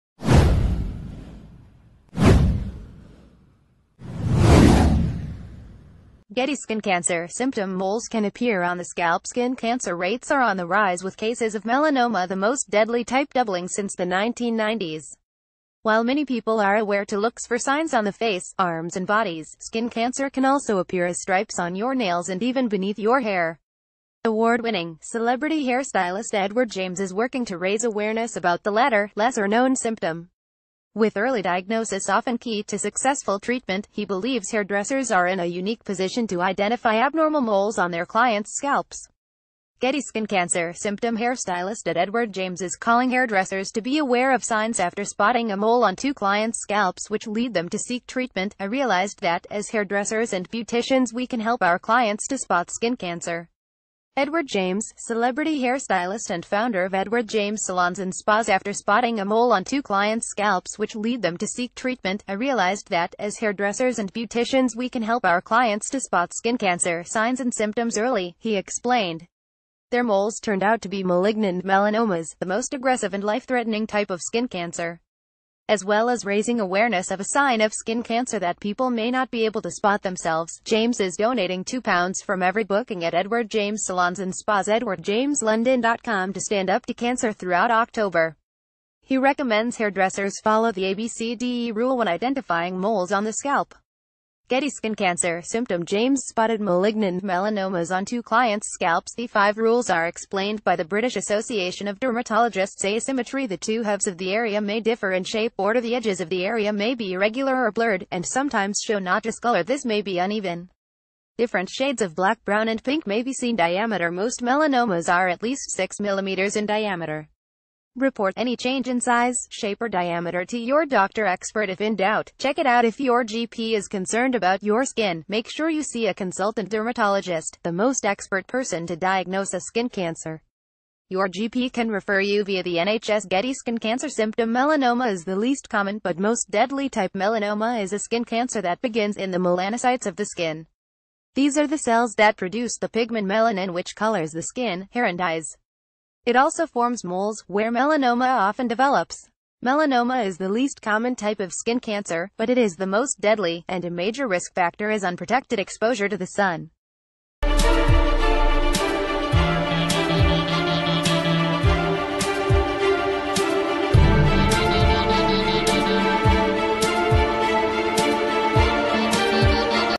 Breaking News | Skin cancer symptoms: four ways to spot deadly melanoma on the scalp
Skin cancer symptoms: four ways to spot deadly melanoma on the scalp

GETTYSkin cancer symptom Moles can appear on the scalpSkin cancer rates are on the rise with cases o...